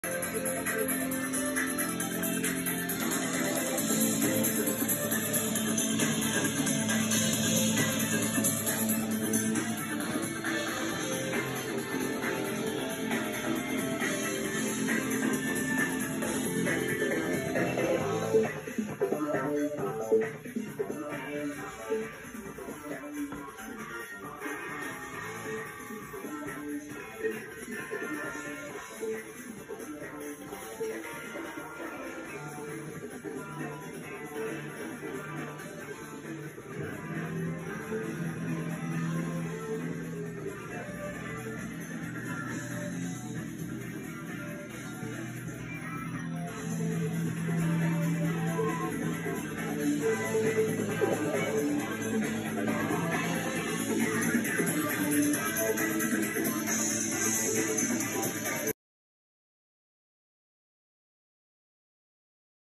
Arcade1up & Home Arcade Tour #23!
Gotham City Arcade is doing a walk throughout the home arcade community, who share a common interest in home arcade space, which are non-coin-operated video games that are typically found in private spaces. Home Arcade community can be online or offline, and they can engage in various activities such as playing, discussing, collecting, or creating arcade games.